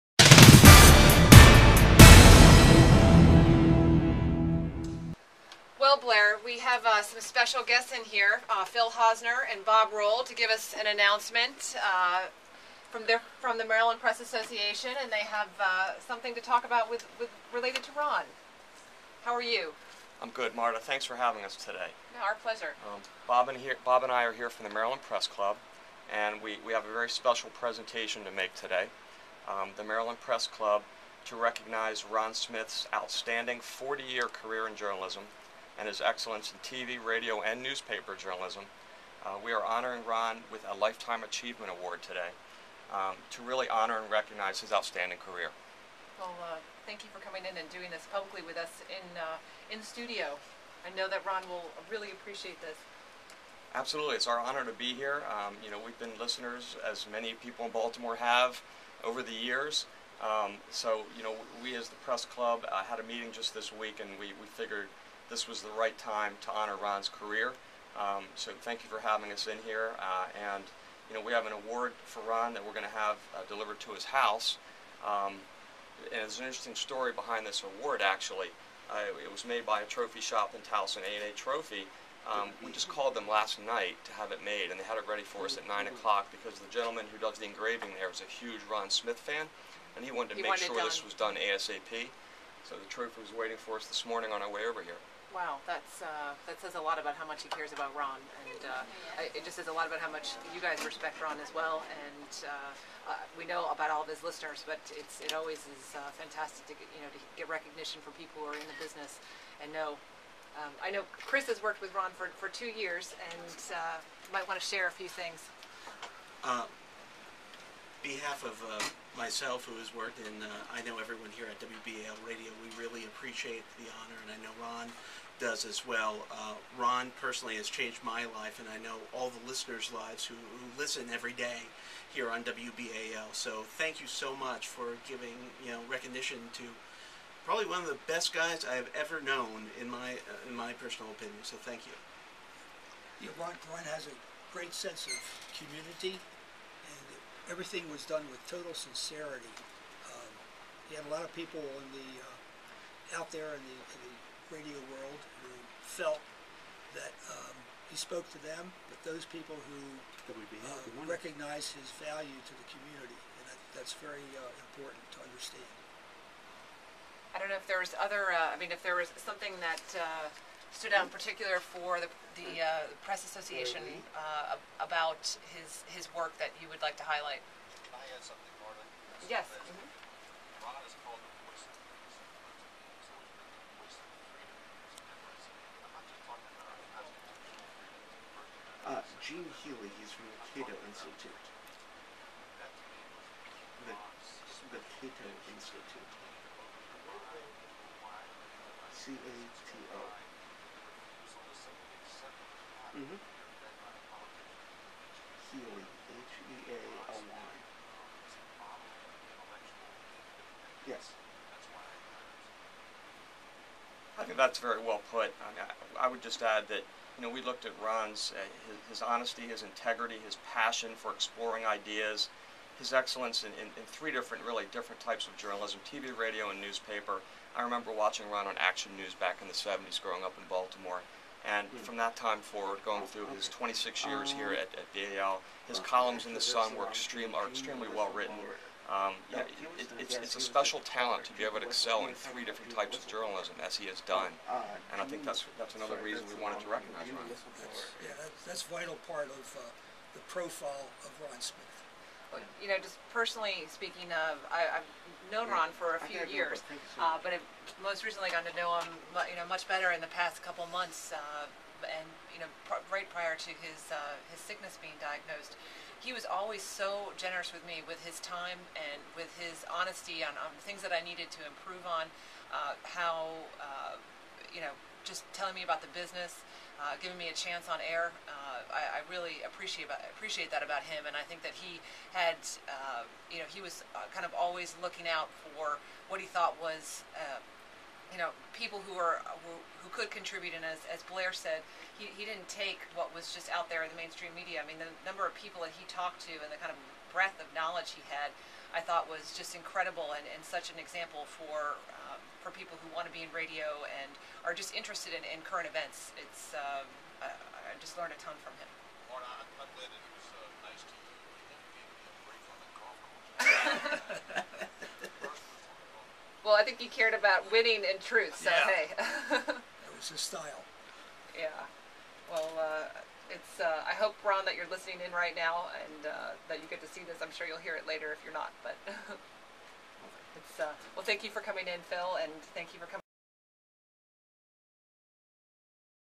Lifetime Achievement Award for Ron Smith
The Maryland Press Club presented its Lifetime Achievement Award to WBAL's Ron Smith, who is battling pancreatic cancer.